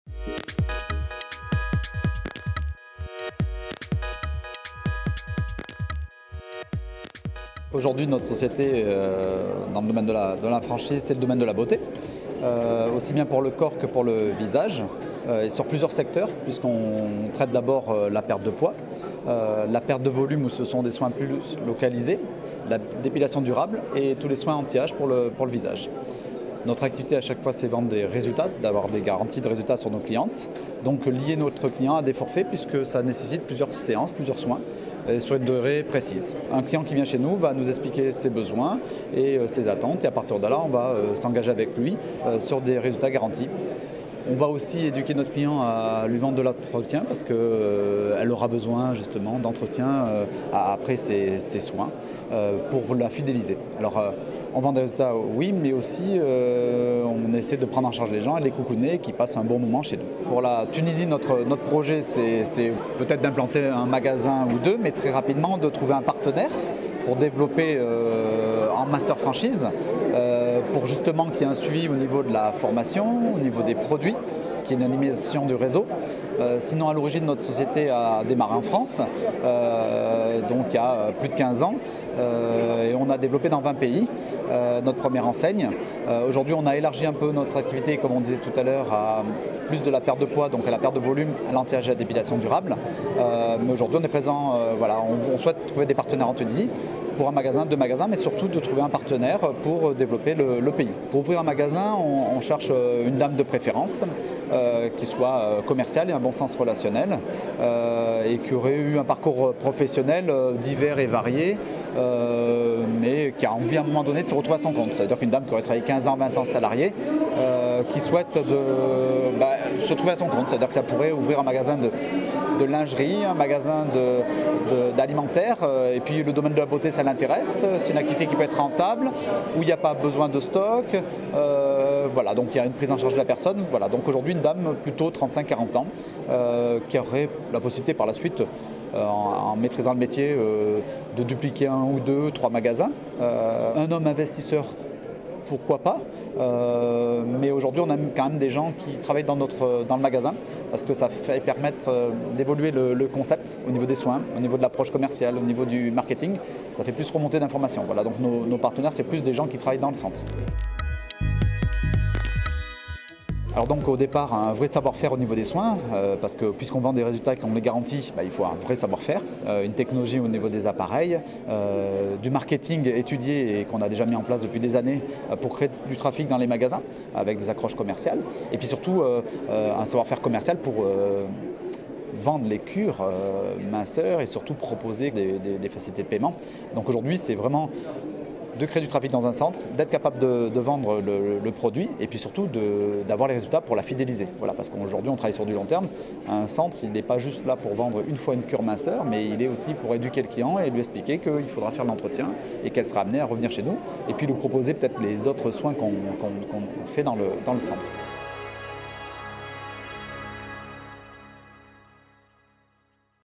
Présentation de la franchise Relooking (Tunisie)
La franchise française Relooking propose à ses clients des soins pour le corps et le visage.

Découvrez le projet de développement de la franchise Relooking en Tunisie et les profils des partenaires et/ ou du master franchisé recherchés.

Interview de Grégory Olagnon (Gérant de la franchise Relooking)